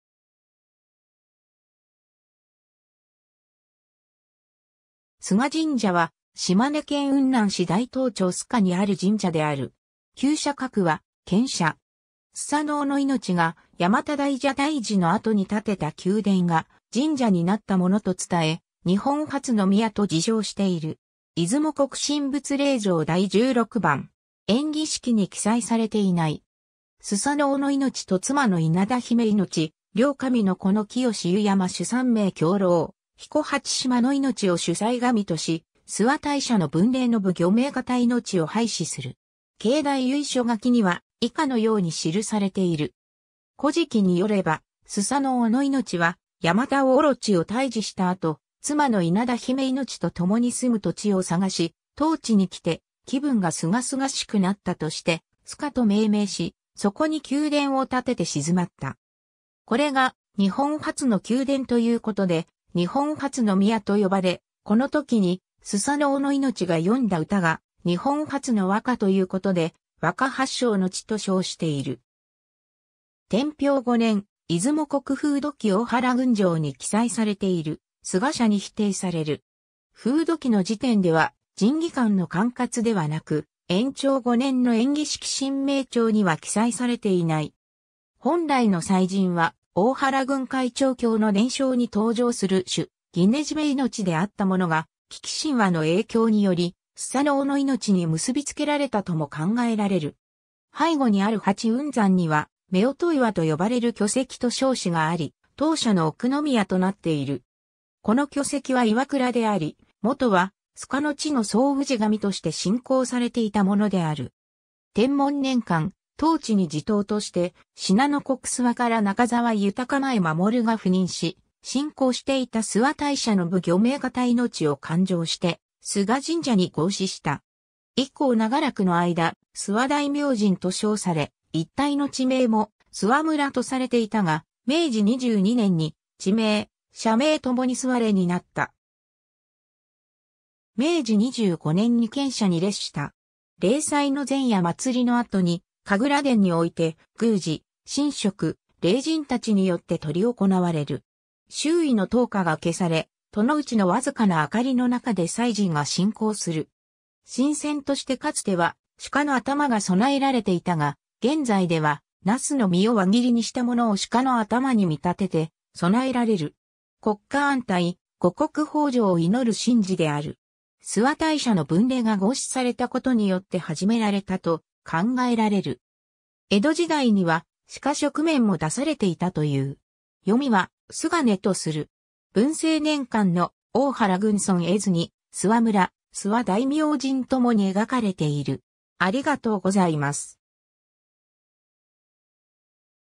須我神社
須我神社（すがじんじゃ）は、島根県雲南市大東町須賀にある神社である。
須佐之男命が八岐大蛇退治の後に建てた宮殿が神社になったものと伝え、「日本初之宮（にほんはつのみや）」と自称している。
出雲國神仏霊場第十六番。
 延喜式に記載されていない。
 須佐之男命と妻の稲田比売命、両神の子の清之湯山主三名狭漏彦八島野命（すがのゆやまぬしみなさろひこやしまのみこと。八島士奴美神）を主祭神とし、諏訪大社の分霊の武御名方命を配祀する。
 境内由緒書きには以下のように記されている。
 『古事記』によれば、須佐之男命は八岐大蛇を退治した後、妻の稲田比売命とともに住む土地を探し、当地に来て「気分がすがすがしくなった」として「須賀」と命名し、そこに宮殿を建てて鎮まった。
これが日本初の宮殿ということで「日本初之宮」と呼ばれ、この時に須佐之男命が詠んだ歌が日本初の和歌ということで、「和歌発祥の地」と称している。
 天平5年（733年）、『出雲国風土記』大原郡条に記載されている「須我社」に比定される。
風土記の時点では神祇官の管轄ではなく、延長5年（927年）の延喜式神名帳には記載されていない。
本来の祭神は大原郡海潮郷の伝承に登場する須義禰命であったものが、記紀神話の影響により須佐之男命に結び付けられたとも考えられる。
 背後にある八雲山には、夫婦岩と呼ばれる巨石と小祠があり、当社の奥宮となっている。
この巨石は磐座であり、元は須賀の地の総氏神として信仰されていたものである。
 天文年間（1532年 – 1554年）、当地に地頭として信濃国諏訪から中沢豊前守が赴任し、信仰していた諏訪大社の武御名方命を勧請して須我神社に合祀した。
以降長らくの間「諏訪大明神」と称され、一帯の地名も「諏訪村」とされていたが、明治22年（1889年）に地名・社名ともに須我になった。
明治25年（1892年）に県社に列した。
 例祭の前夜祭の後に神楽殿において宮司、神職、伶人（雅楽を演奏する者）たちによって執り行われる。
周囲の灯火が消され、殿内のわずかな明かりの中で祭事が進行する。
神饌としてかつては鹿の頭が供えられていたが、現在では茄子の実を輪切りにしたものを鹿の頭に見立てて供えられる。
国家安泰、五穀豊穣を祈る神事である。
諏訪大社の分霊が合祀されたことによって始められたと考えられる。
江戸時代には鹿食免も出されていたという。
 ^ 読みは「すがね」とする（加藤義成『出雲国風土記参究』）。
 ^ 文政年間の『大原郡村々絵図』に諏訪村、諏訪大明神ともに描かれている。